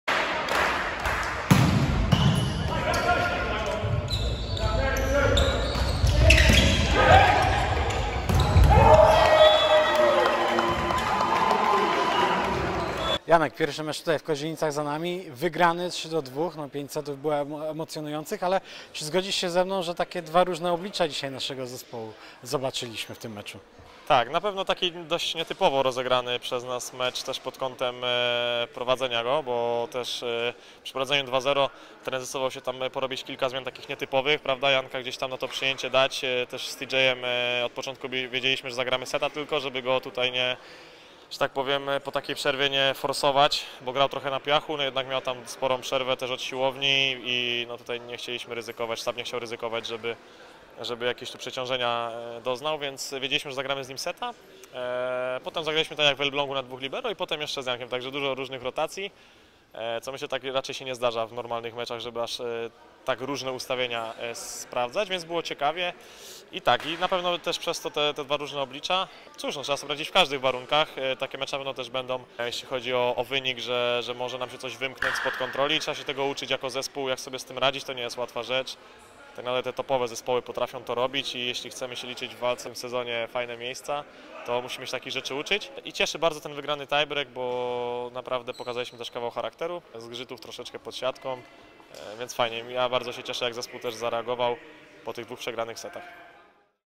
Jan Firlej: Pokazaliśmy kawał charakteru (sparing z LUK Politechniką Lublin, 10.09)
- Cieszy wygrany tie-break, bo pokazaliśmy kawał charakteru. Było także trochę zgrzytów pod siatką. Cieszę się także ze sposobu, w jaki nasz zespół zareagował po dwóch przegranych setach - tak pierwszy mecz w Kozienicach podsumował Jan Firlej 🎥